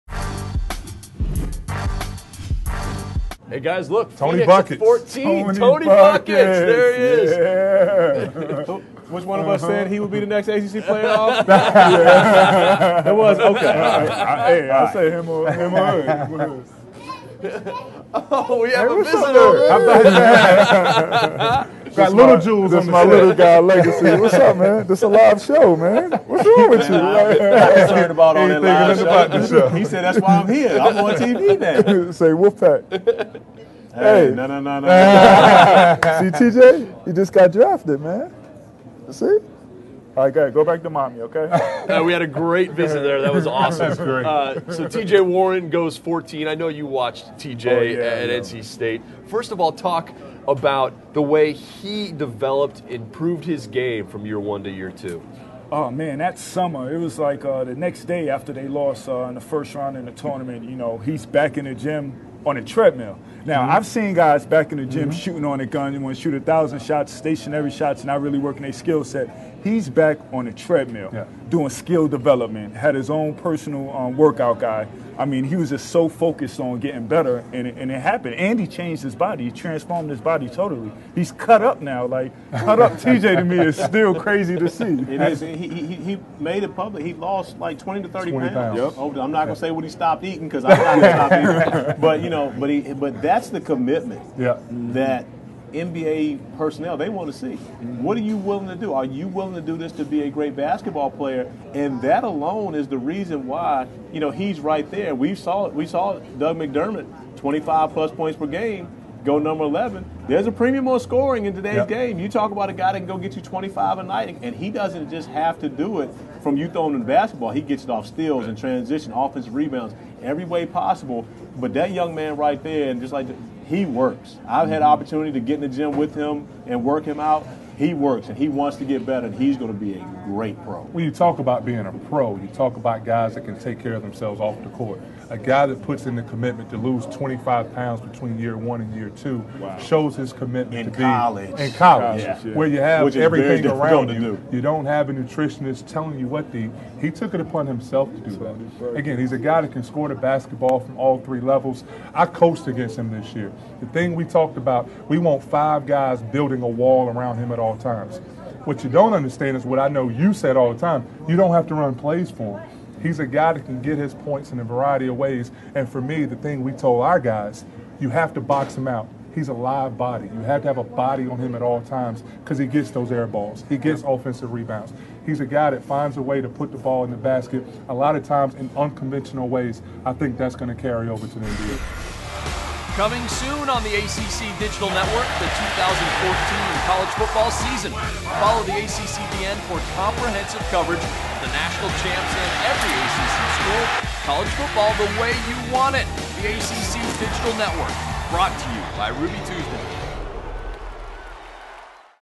Julius Hodge's Son Celebrates T.J. Warren Being Selected By Suns
Former NBA Draftees Julius Hodge and Cory Alexander and former UNC player Jason Capel discuss NC State sharpshooter T.J. Warren TonyBuckets being selected 14th overall by the Phoenix Suns while hanging out at the ACC Digital Network Players Draft Party. Watch as Julius even gets a visit from an excited little guest!